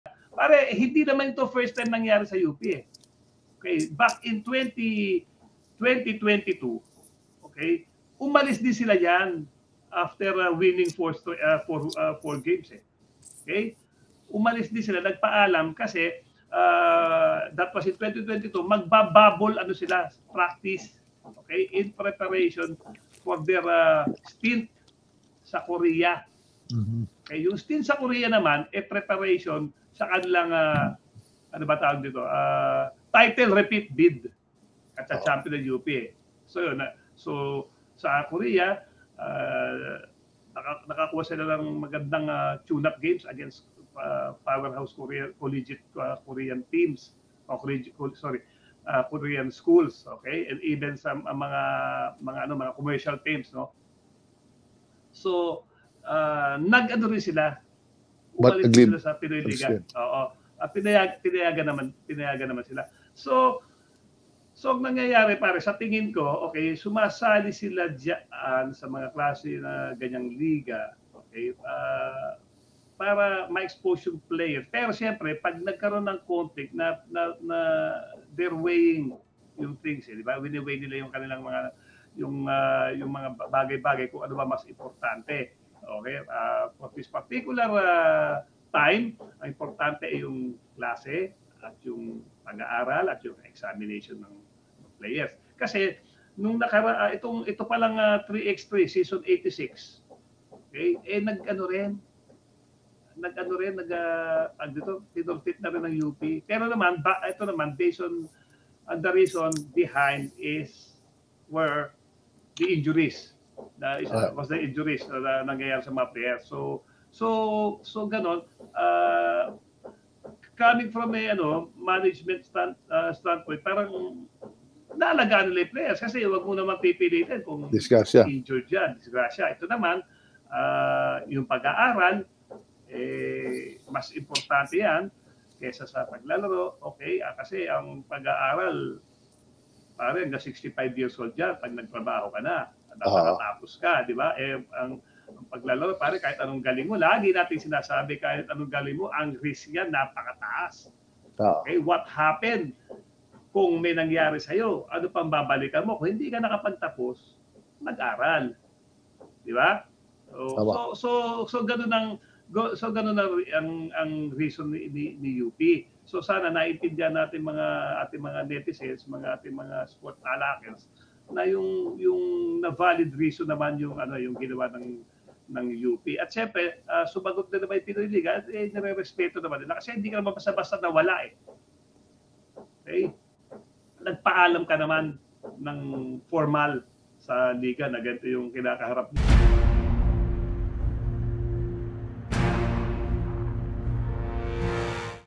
Pagsibat ng Fighting Maroons sa Pinoyliga Cup hindi na bago - Esplana
Nagbalik tanaw si ‘Mr. Cool’ Gerry Esplana sa UP Fighting Maroons kung saan noong 2022 umano ay nag-backout din ito sa Pinoyliga Cup. Alamin ang kuwento!